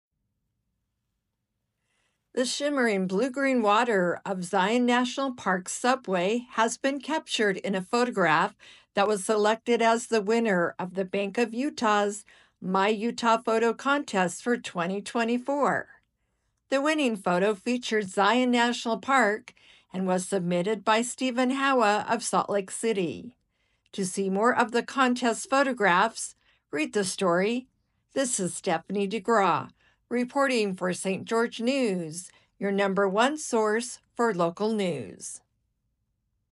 ‘The camera gets me out’: Utah man wins 1st place for photo of Zion’s Subway in annual contest
ST. GEORGE — The shimmering blue-green water of Zion National Park’s Subway was captured in a photograph selected as the winner of the Bank of Utah’s “My Utah“ Photo Contest for 2024. Written by Stephanie DeGraw